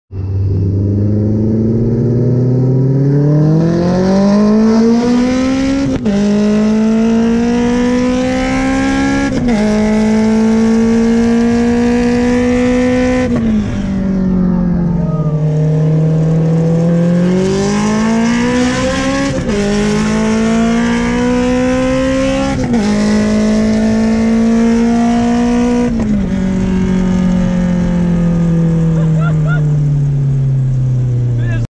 270hp All Motor K20 vs Challenger RT
270hp All Motor Civic EK Hatch vs Challenger RT's
V8's vs 4cyl
 Brainerd International raceway.
RSX head, Drag Cartel Cams, Supertech Performance springs attached to a TSX block.

Go pro used for filming.